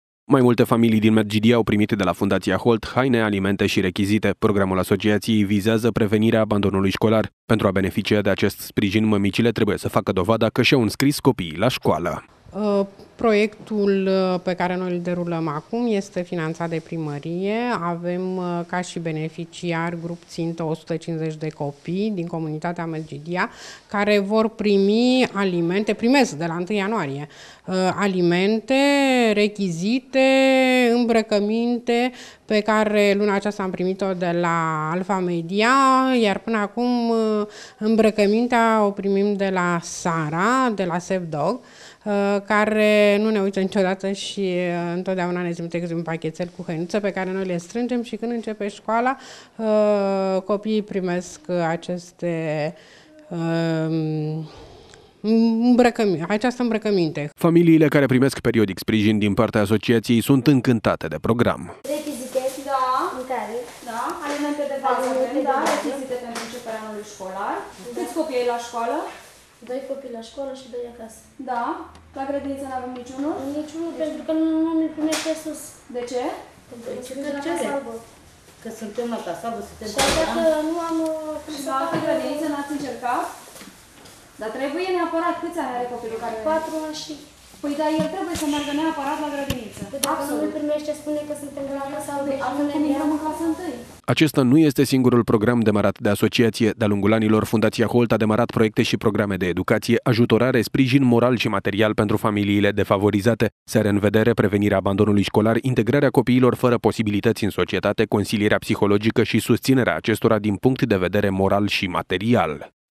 RECHIZITE ŞI HĂINUŢE PENTRU COPII
Începutul anului şcolar bate la uşă! Şi ca de fiecare dată FUNDAŢIA HOLT sprijină familiile cu posibilităţi limitate prin oferirea de hăinuţe, alimente şi rechizite. Totul pentru a preveni abandonul şcolar dar şi pentru a reuşi integrarea în societate a copiilor defavorizaţi.